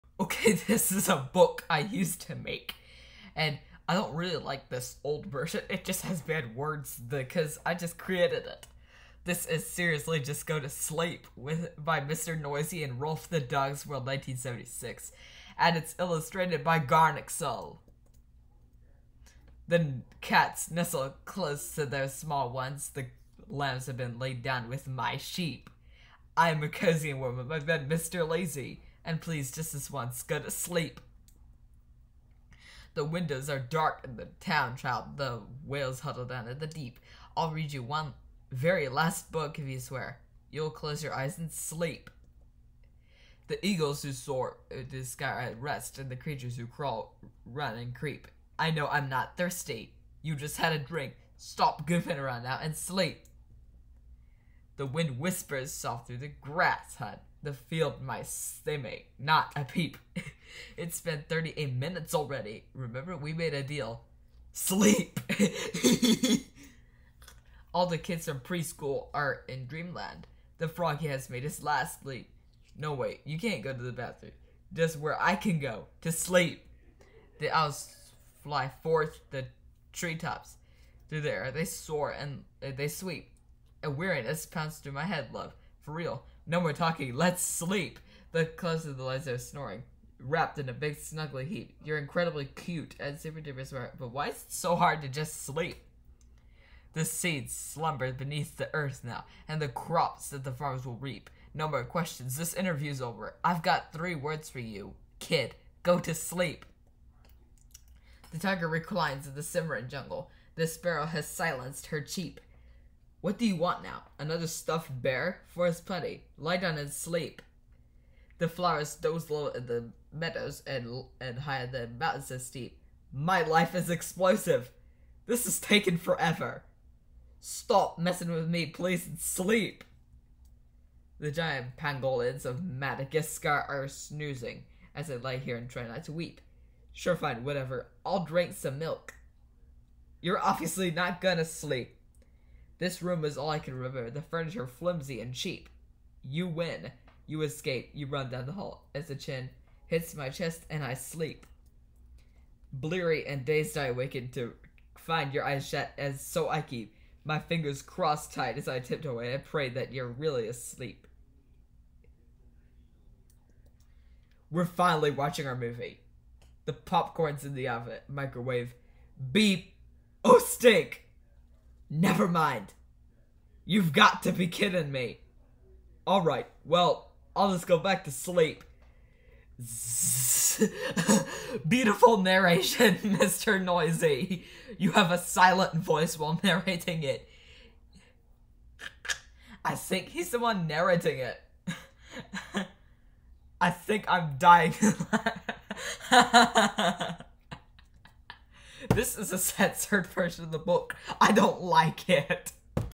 Seriously Just Go To Sleep (Mr. Men and Little Misses Version)
I'll buy that book soon and I'll make Mr. Noisy read it.
Narration: Mr. Noisy
Book created by me
Mr. Noisy voice provided by Richard Epcar